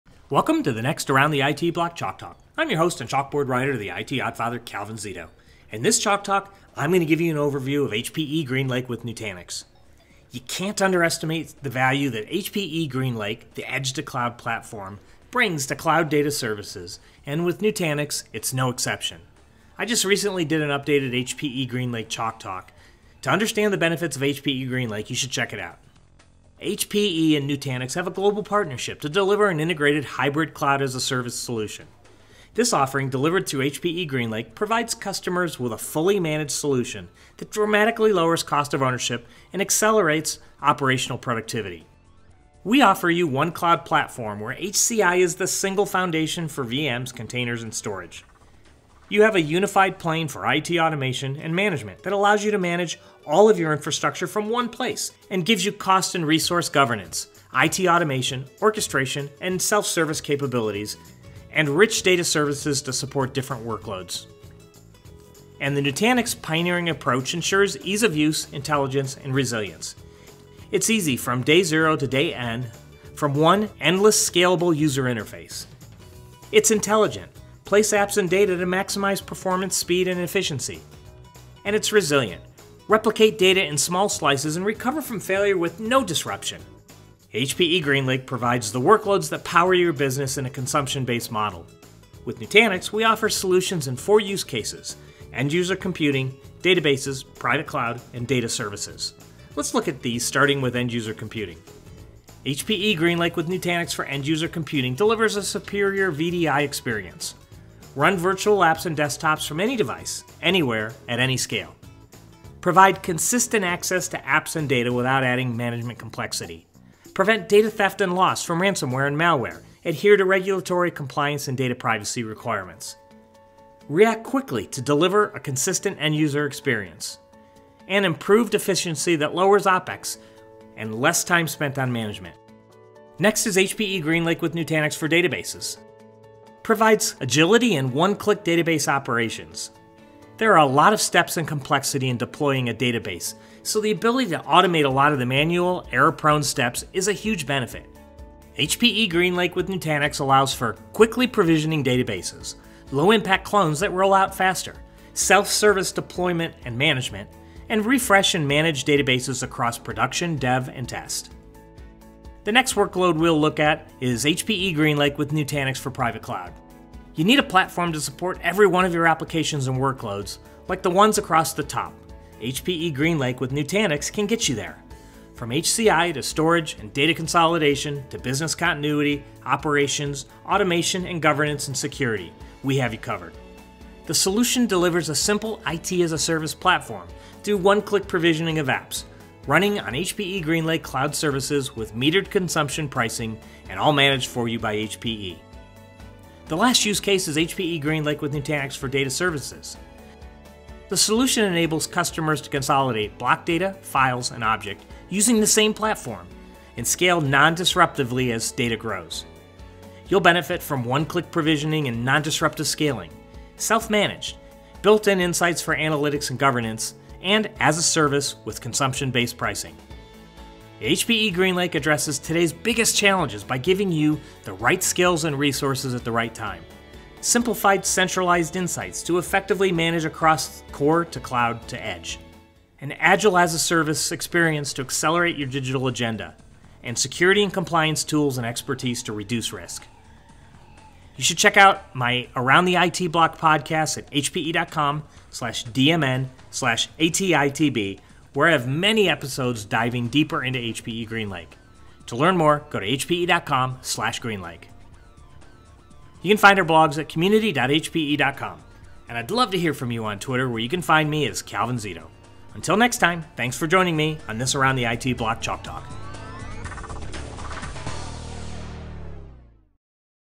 HPE GreenLake with Nutanix | Chalk Talk
HPE and Nutanix have a global partnership to deliver an integrated hybrid cloud as-a-Service solution. This offering delivered through HPE GreenLake, provides customers with a fully managed solution that dramatically lowers cost of ownership and accelerates operational productivity. Learn about the solution in this Chalk Talk.